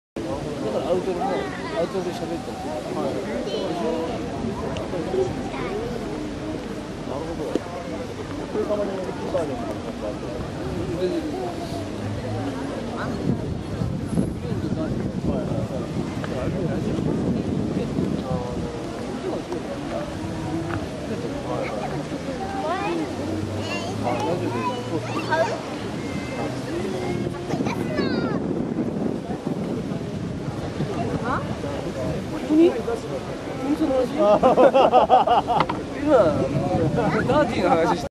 三笠公園にある戦艦三笠
２０１３年３月に横須賀にある三笠公園にて撮影。
スプリングフェスタの中撮影しました。